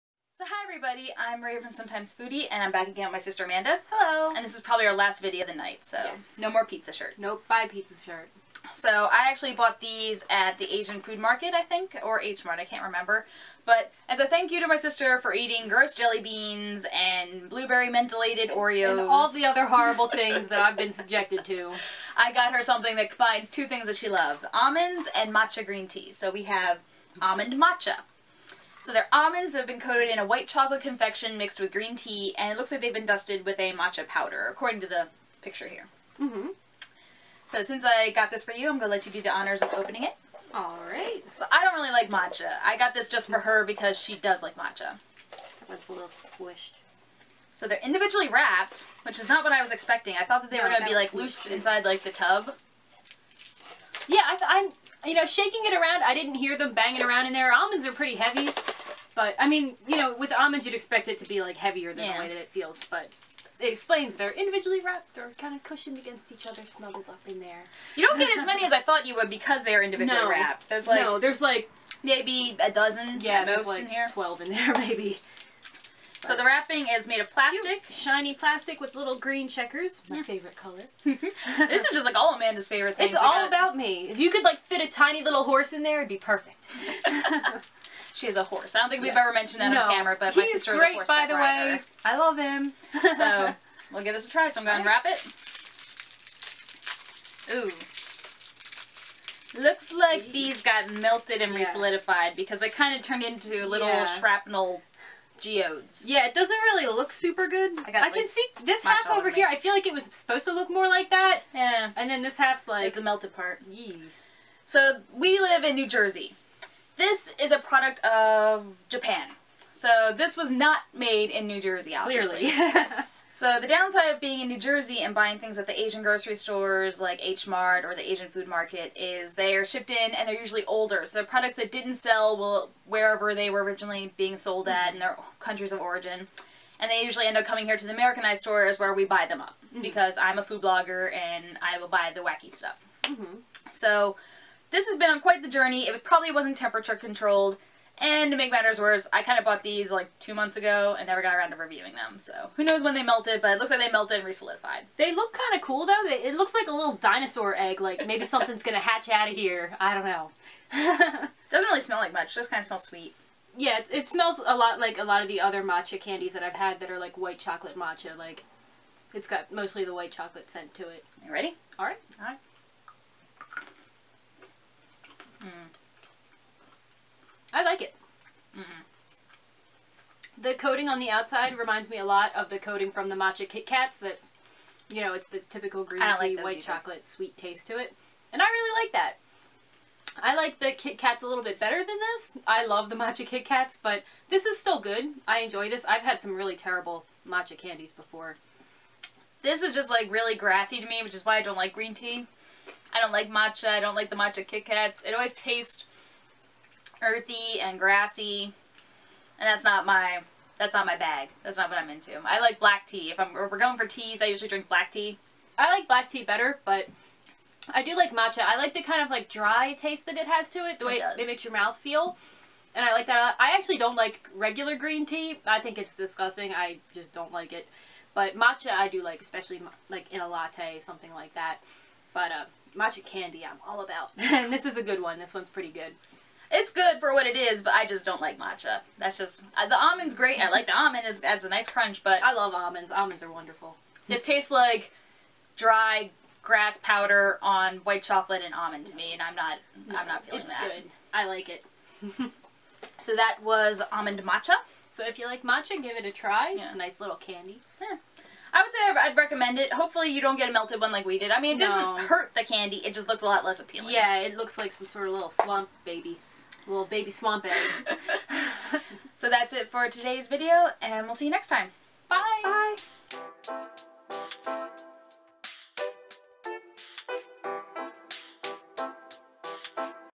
Matcha Almonds Review
Here's something I know Amanda will like for sure to thank her for eating barf flavored Jelly Beans with me.